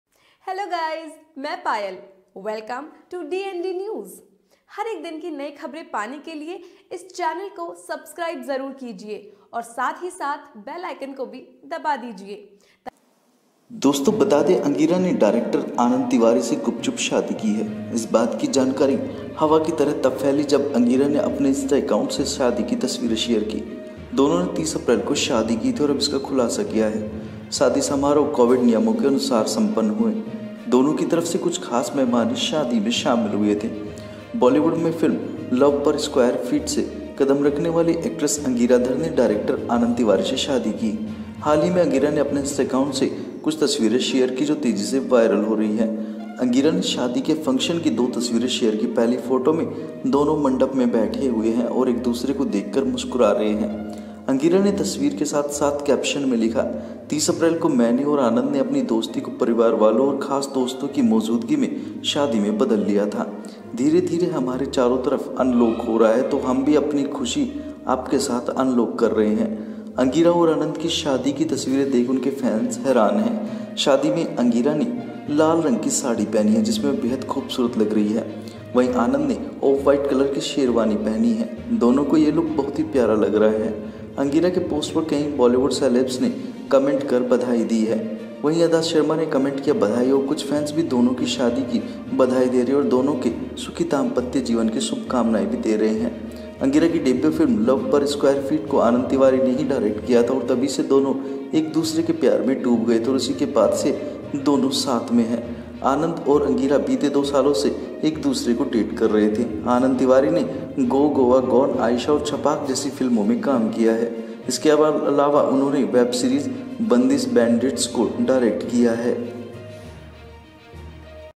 यामी गौतम के बाद अब इस मशहूर हीरोइन ने रचाई डायरेक्टर के साथ गुपचुप शादी, देखिए सबसे पहली तस्वीरें
Tell me Angira has secretly married director Anand Tiwari. The information spread like air when Angira shared wedding pictures from her Instagram account. The two got married on April 30 and have now revealed it. The wedding ceremony was held as per the Covid rules. Only a few special guests from both sides attended the wedding. Actress Angara Dhar, who made her Bollywood debut with 'Love Par Square Foot', is married to director Anand Tiwari. Angira recently shared some pictures from her Instagram account which are going viral fast. Angira shared two pictures from the wedding function. In the first photo, the two are sitting in the mandapa and smiling at each other.  Angira wrote in the picture as well as the caption, 'On April 30, Anand and I turned our friendship into a wedding in the presence of family and special friends. Slowly unlocking around us so we are also unlocking our happiness with you. '
Angira and Anand's fans are shocked to see the wedding pictures. Angira is wearing a red saree at the wedding in which she looks gorgeous. Anand is wearing an off-white sherwani. The two are loving this look. Many Bollywood celebs have commented on Angira's post and congratulated her. Actress Shorvari commented: 'Dada has become lucky. A lot of love to both of you'. Ada Sharma commented, 'Congratulations'. Some fans are also wishing the two a happy married life and wishing them a happy married life. Angira's debut film 'Love Par Square Foot' was directed by Anand Tiwari and since then the two have fallen in love with each other and the two have been together since then. Anand and Angara had been dating each other for the past two years. Anand Tiwari has acted in films like 'Go Goa Gone', 'Ayesha' and 'Chhapak'. He has also directed the web series 'Bandish Bandits'.